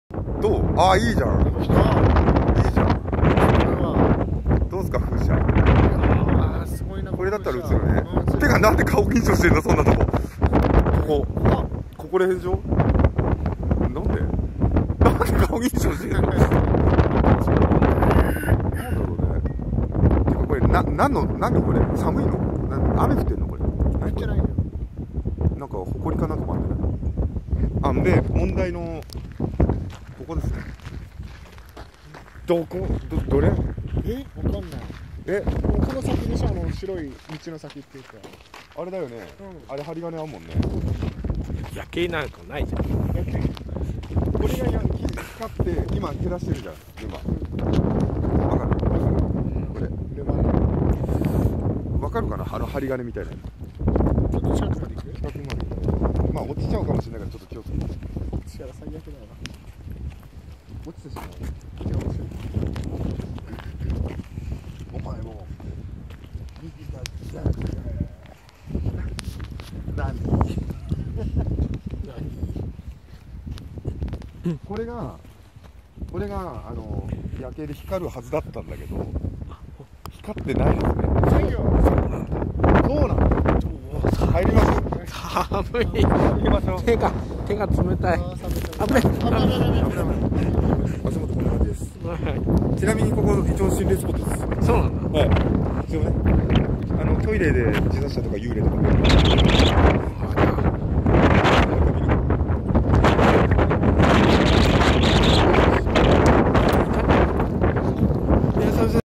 心霊なの？3人で行った霞ケ浦総合公園
前ゼルさんと昼間行った場所です。
夜は夜景がきれいだとと聞いたので行ってみると・・・・

ねぇ・・・